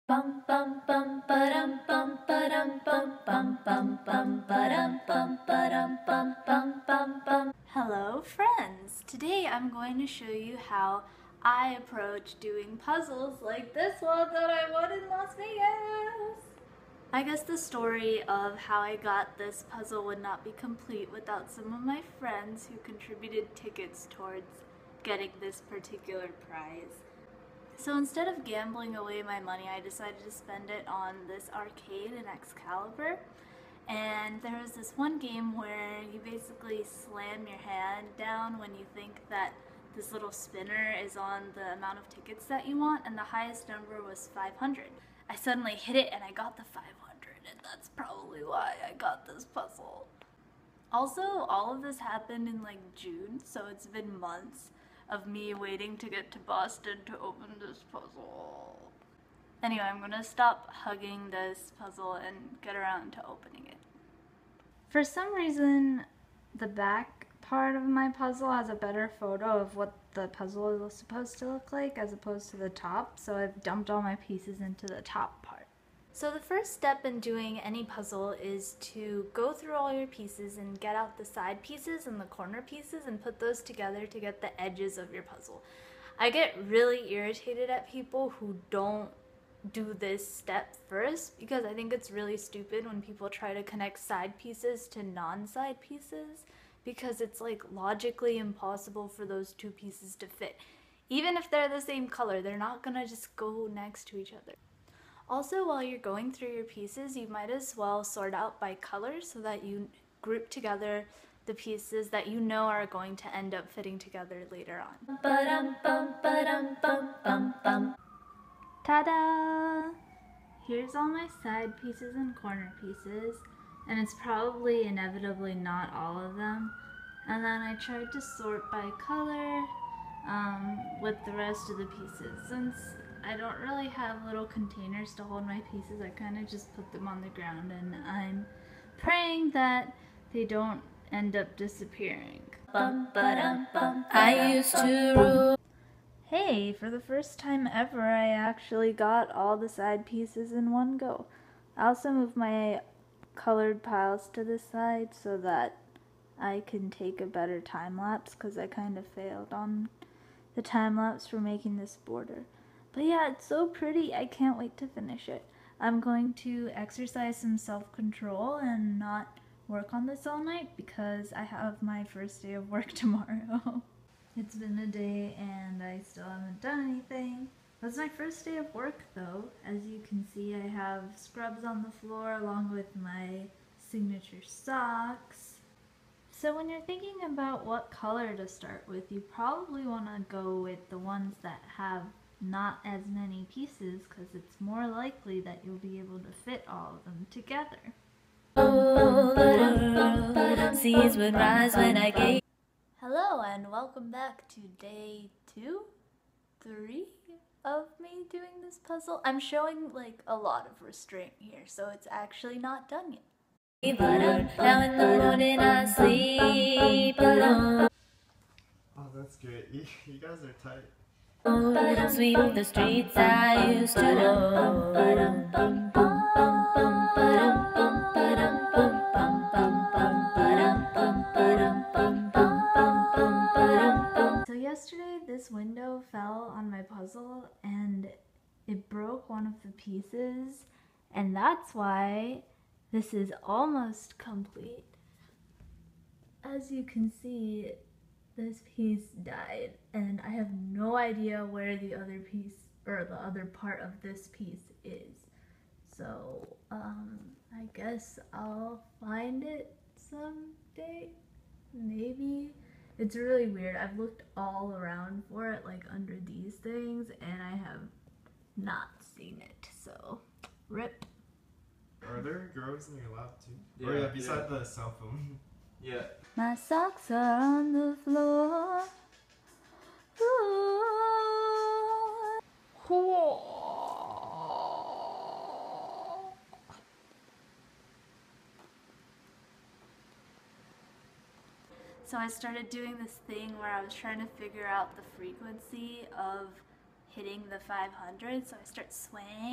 How to do Jigsaw Puzzles
Documentation of me doing this puzzle I've patiently waited to do for months.